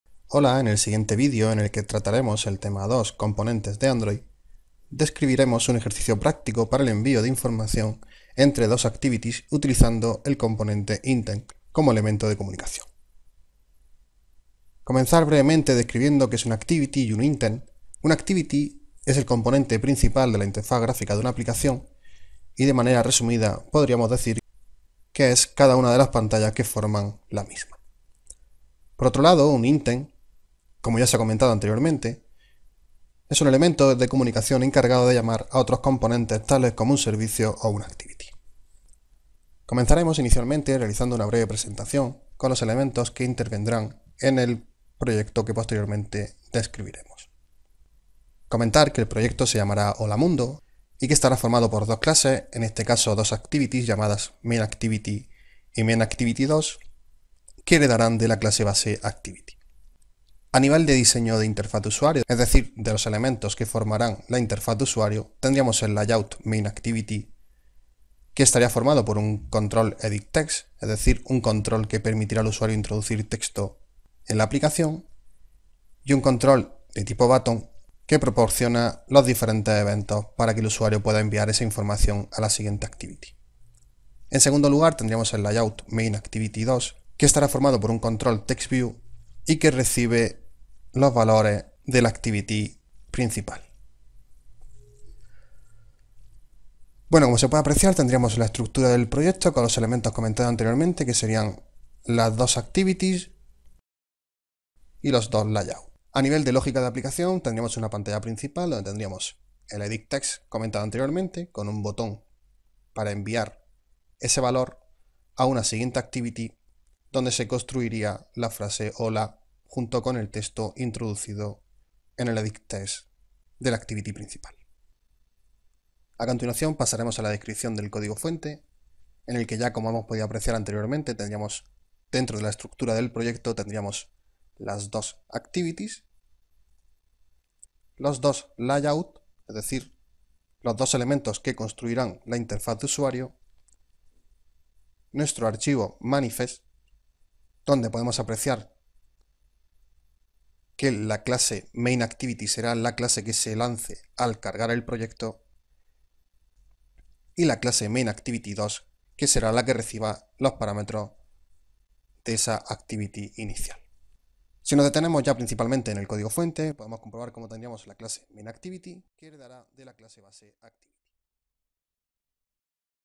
Curso Android Studio Tema2: componentes (inicio)
Explicamos una aplicación estilo 'Hola Mundo' con dos activities y un Intent. Puedes ver el video completo en nuestra web: